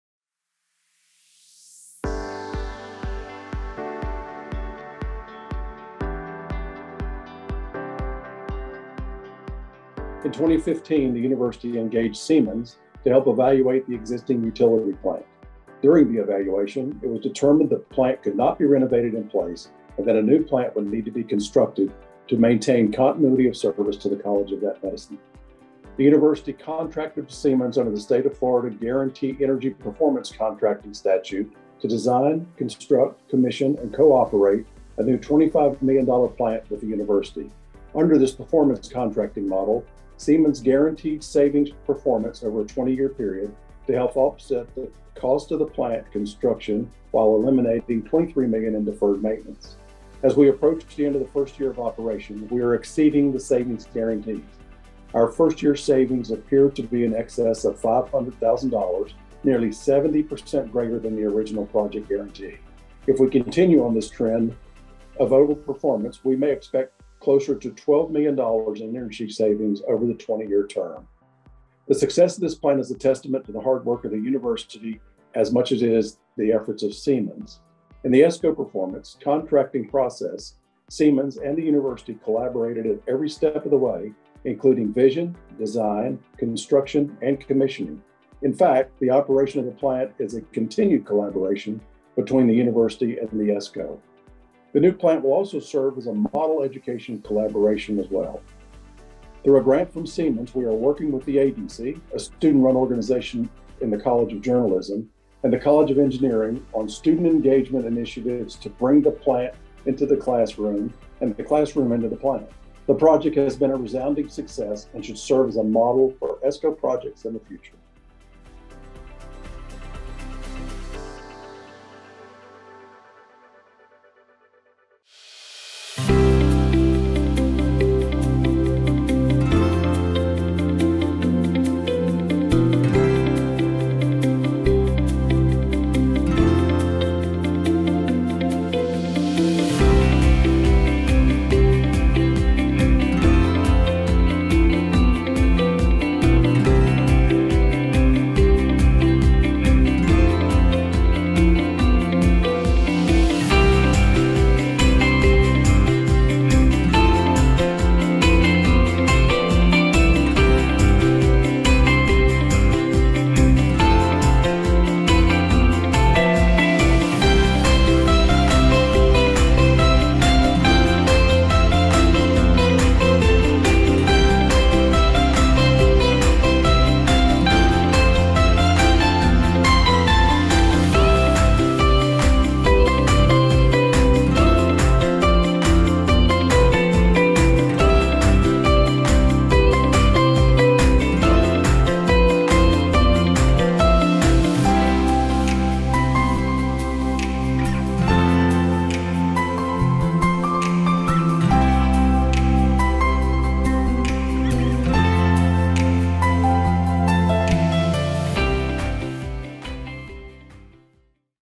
BOT Construction Update December 2021
This is a Board of Trustees Construction update video showcasing numerous projects in progress (December 2021).